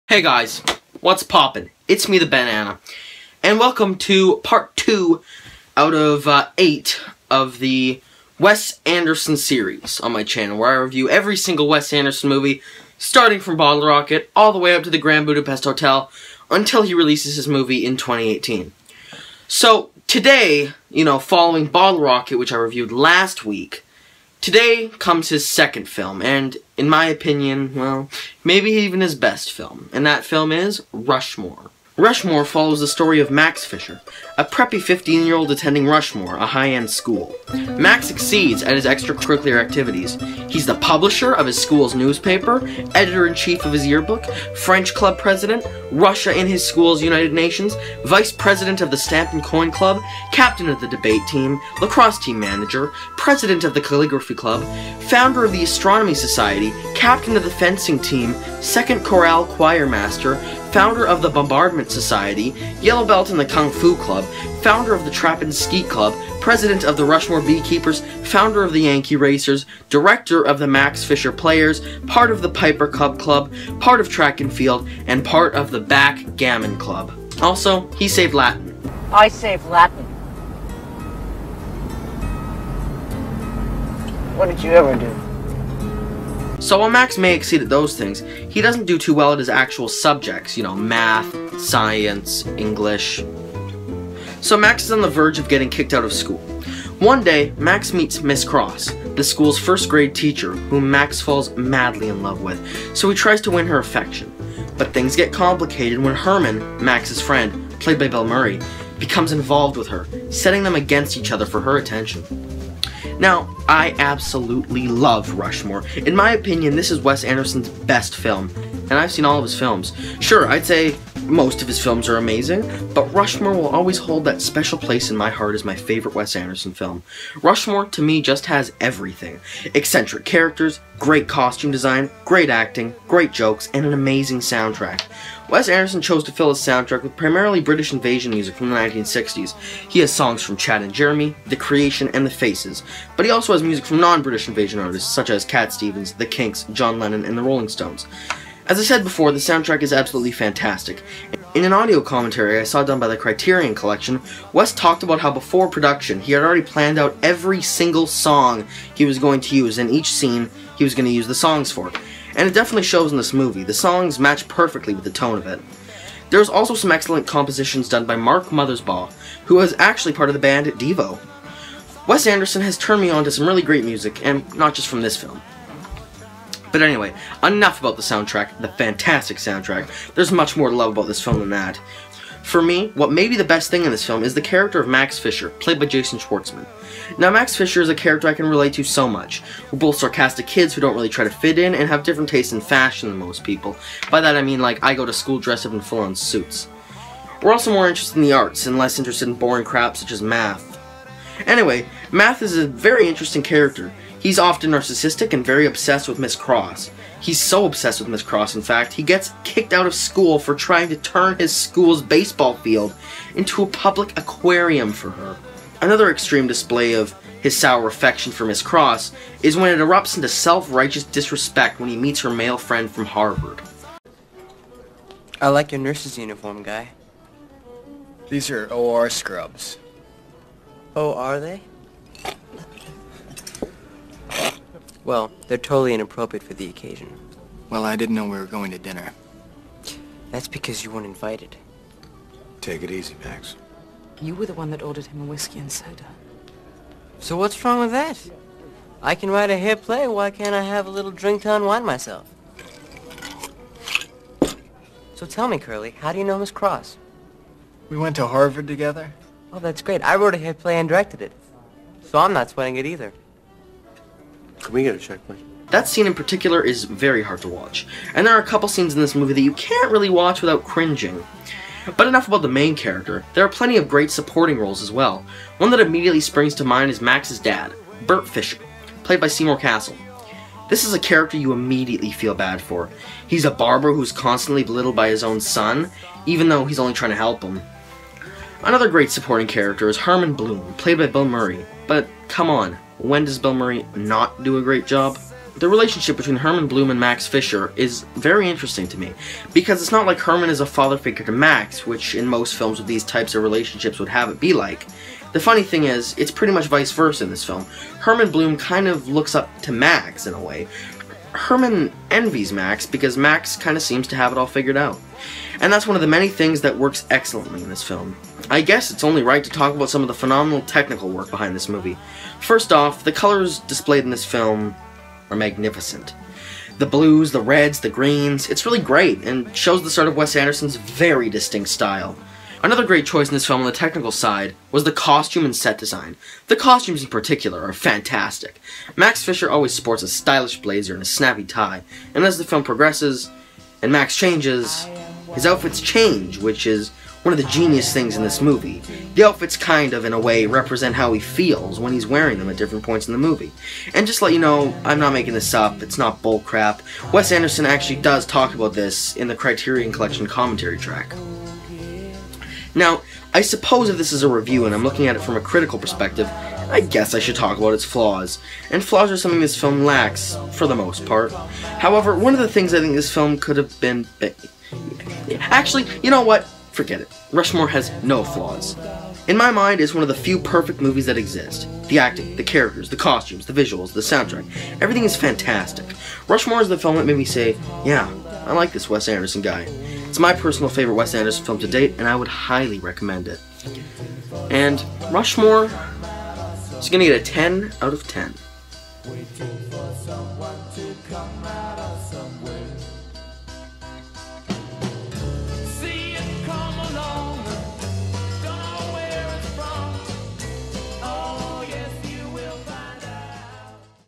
Rushmore (1998) REVIEW
Hey guys, it's the Benana, and today you're watching the second installment of my Wes Anderson reviews. In this episode, I will be looking at Wes Anderson's second feature, Rushmore from 1998. Enjoy!